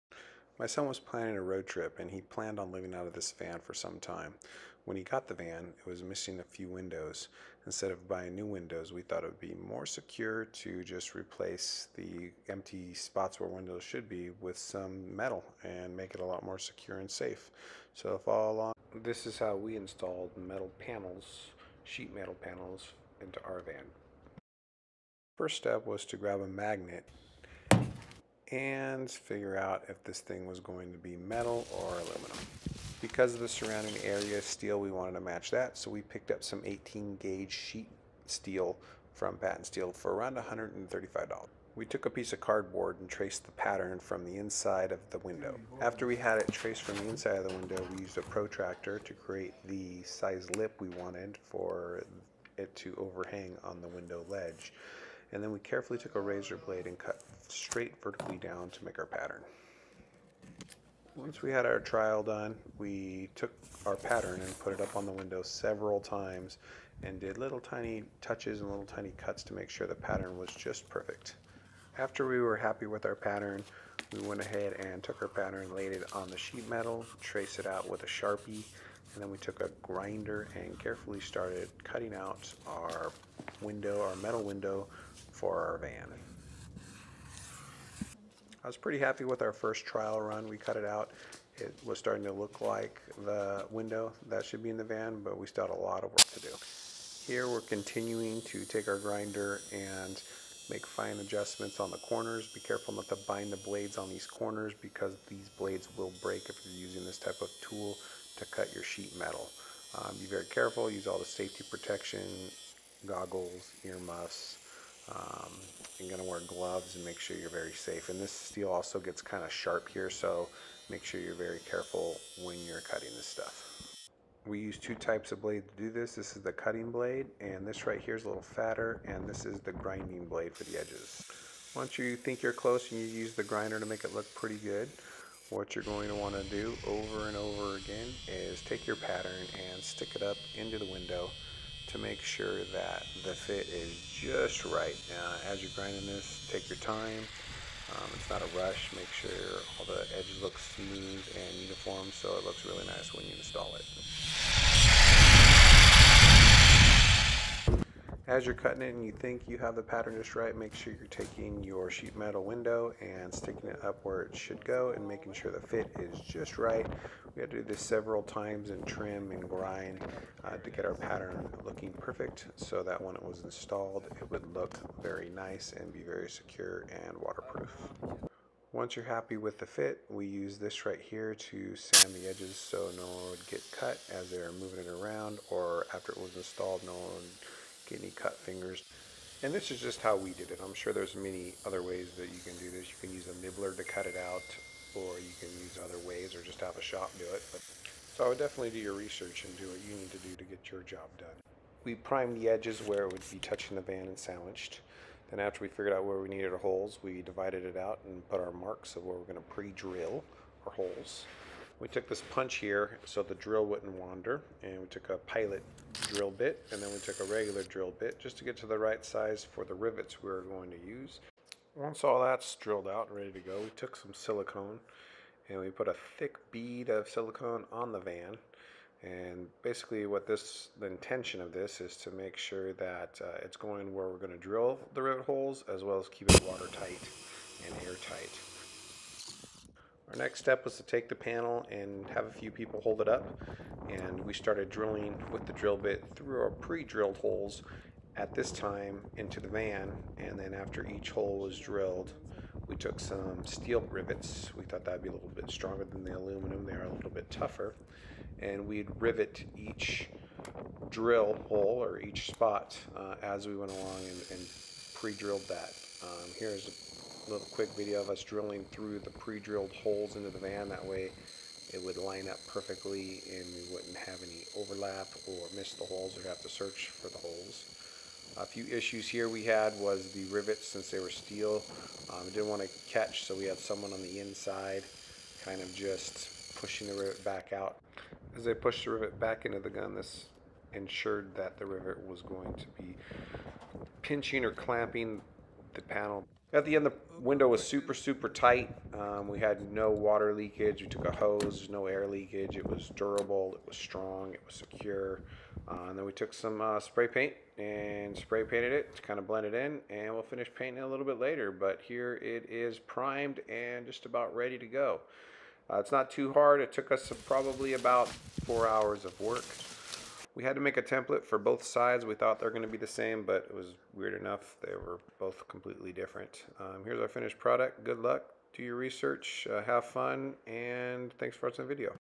Changing or adding Metal Window Panels for a travel van
Our journey to fill in broken windows with strong secure window panels.